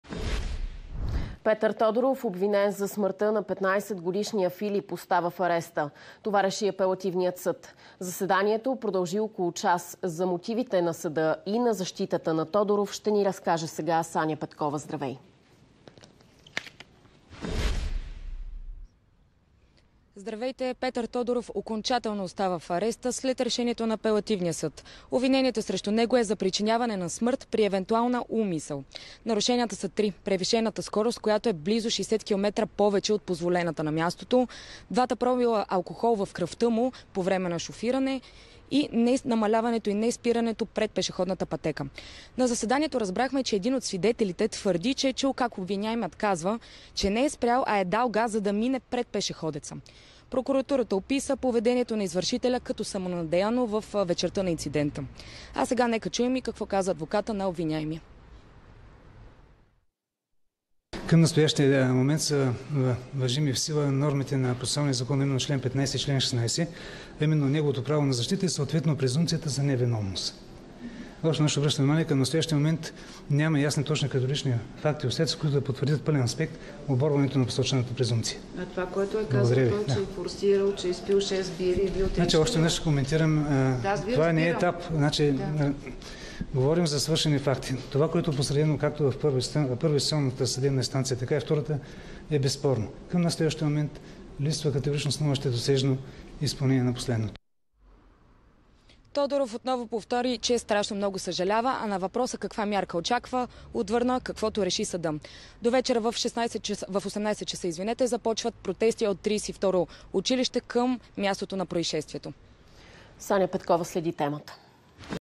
Окончателно оставиха в ареста шофьора, който прегази и уби 15-годишния Филип
Шофьорът Петър Тодоров, който в нетрезво състояние прегази и уби 15-годишния ученик Филип на столичната ул. "Гурко", остава в ареста.

Това постанови окончателно Софийският апелативен съд и така потвърди решението на първата инстанция, въпреки че адвокатът на Тодоров поиска по-лека мярка за неотклонение.